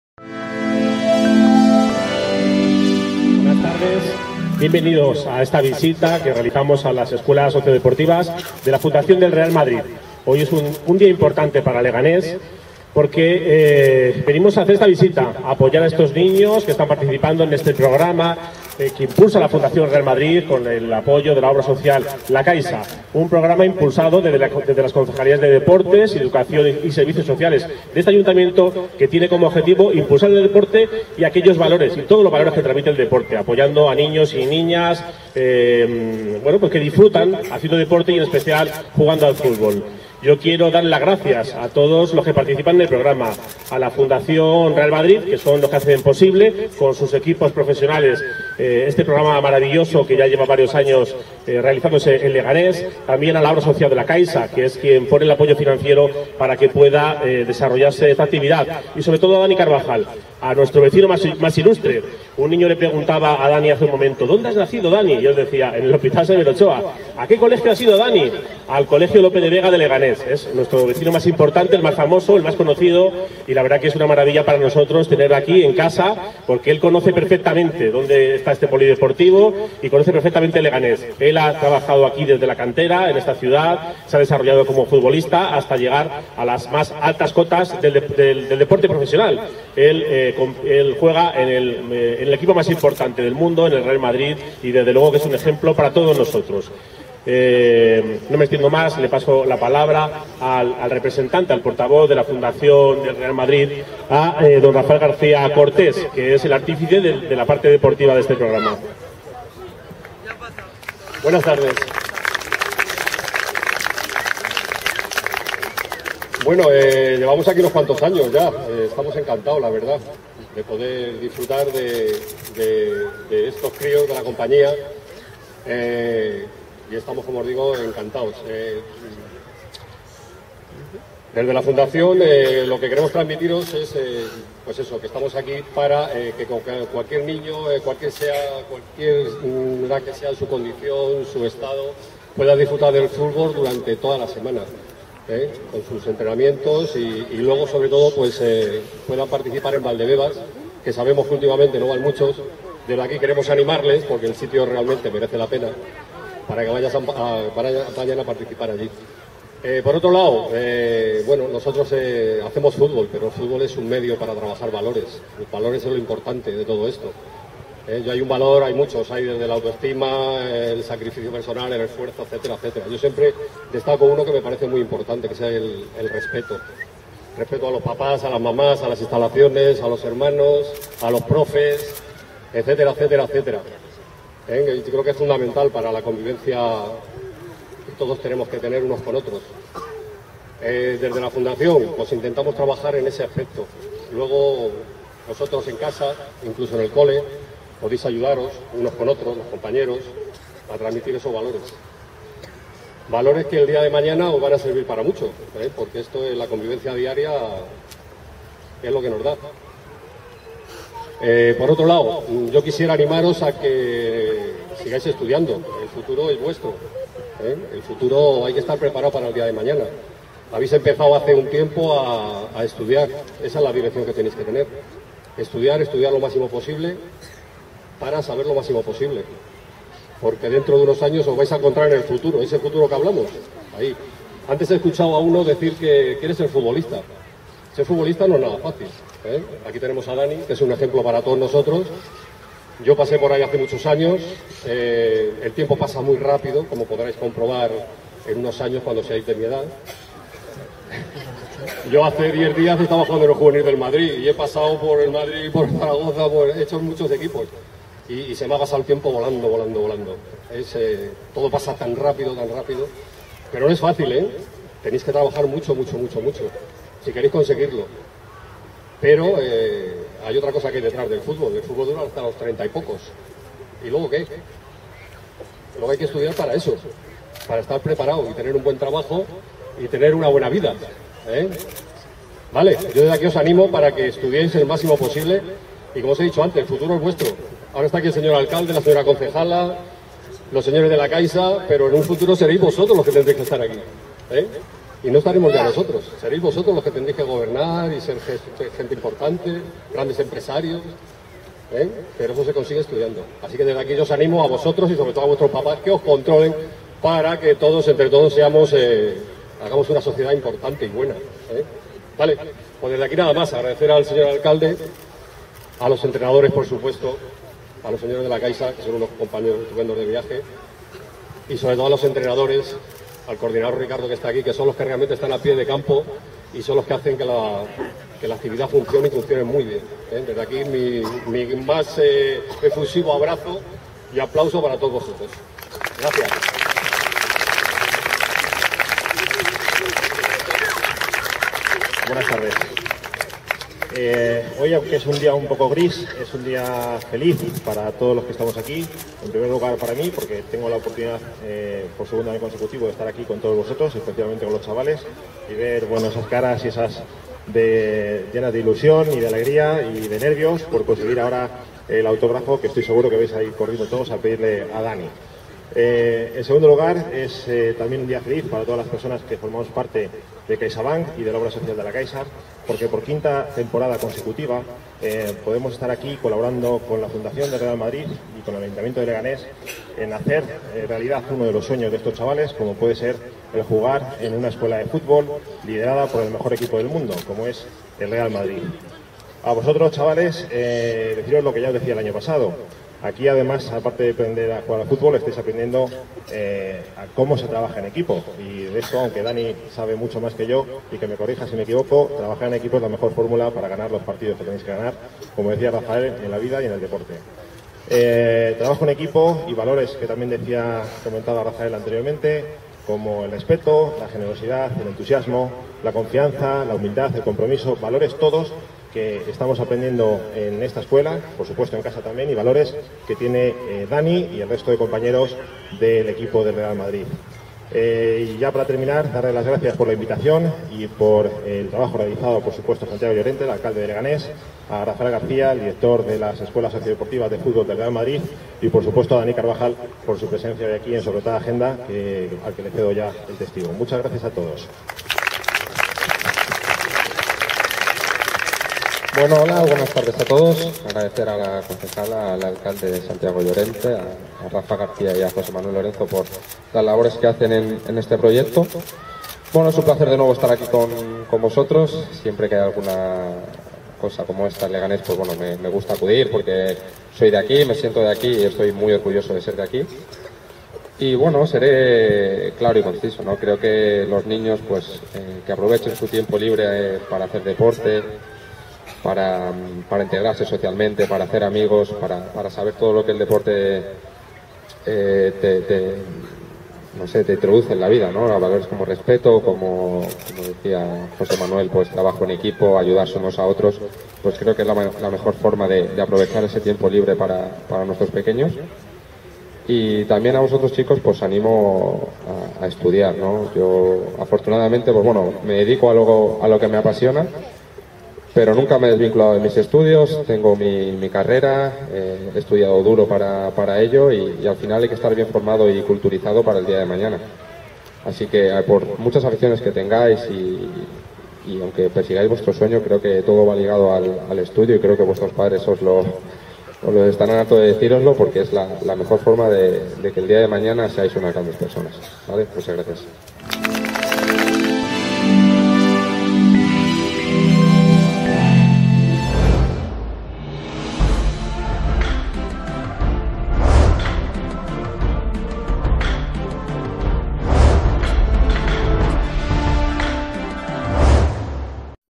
DANI CARVAJAL VISITÓ UNA ESCUELA DE FÚTBOL EN LEGANÉS
Leganés, 30/1/2017.- El lateral derecho del Real Madrid, Dani Carvajal, volvió a visitar a los alumnos de la escuela sociodeportiva de fútbol para la integración en Leganés, su ciudad natal, donde estuvo acompañado por el alcalde de la localidad, Santiago Llorente, José Manuel Lorenzo, director de Área de Negocio de CaixaBank en la zona de Fuenlabrada, e Israel Alonso, director de Área de Negocio de CaixaBank en la zona de Getafe y el director de escuelas sociodeportivas de la Fundación Real Madrid, Rafael García Cortés.
Por quinta temporada consecutiva, la alianza entre la Fundación Real Madrid y la Obra Social “la Caixa” para la educación en valores a través del deporte, ha hecho posible que casi un centenar de niños y niñas de Leganés, seleccionados por las Concejalía de Servicios Sociales y Deportes, puedan disfrutar del fútbol como vehículo de aprendizaje de valores clave de la convivencia y la formación integral de la persona.
Carvajal señaló la importancia del esfuerzo y saludó a todos los alumnos, mientras el alcalde de Leganés, Santiago Llorente, agradeció a Dani Carvajal “su compromiso permanente con Leganés” y destacó que para la ciudad es un orgullo contar con este ilustre vecino que hace tan sólo unos años jugaba en la liga infantil. El alcalde añadió que “el jugador representa un ejemplo para miles de niños y niñas de Leganés que ven como con esfuerzo y tesón se puede alcanzar cualquier meta”. Por su parte, José Manuel Lorenzo de la Obra Social “la Caixa” destacó la importancia de “trabajar en equipo teniendo siempre presentes unos valores fundamentales: el entusiasmo, el respeto,  la generosidad, el compromiso, la confianza y la humildad”.
Finalmente, el veterano jugador y director de todas las Escuelas de la Fundación, García Cortés, quiso remarcar la importancia de estos proyectos para contribuir a través del fútbol a la educación en los valores y para la integración de todo tipo de colectivos sean cuales sean sus circunstancias y capacidades.